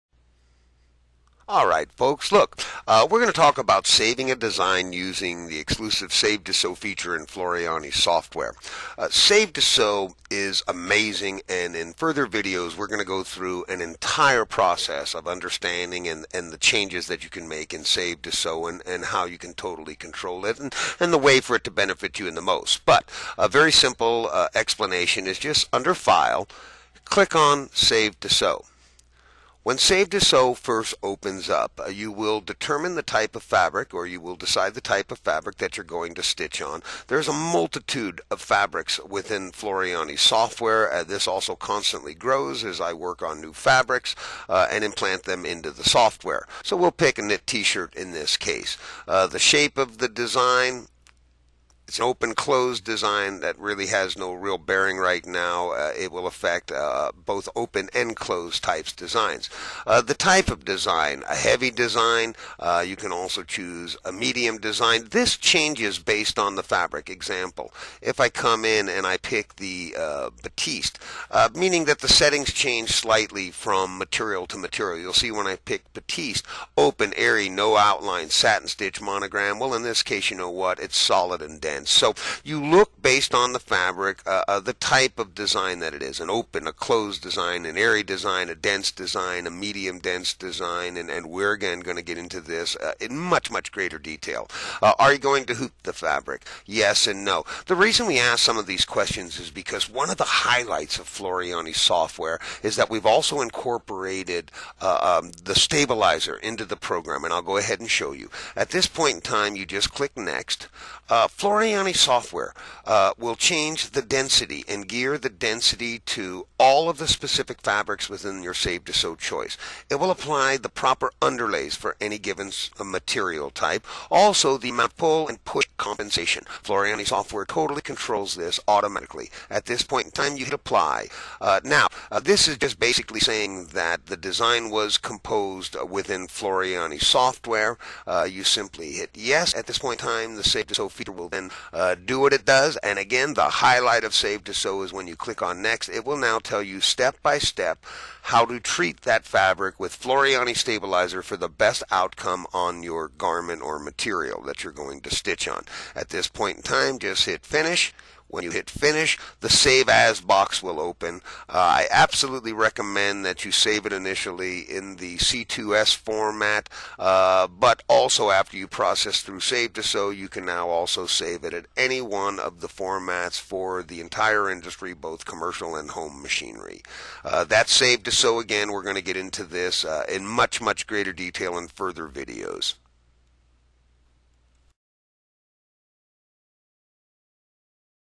Saving a Design Using Save2Sew.wmv
This video shows you how to use the Save2Sew feature of the Floriani Total Control software.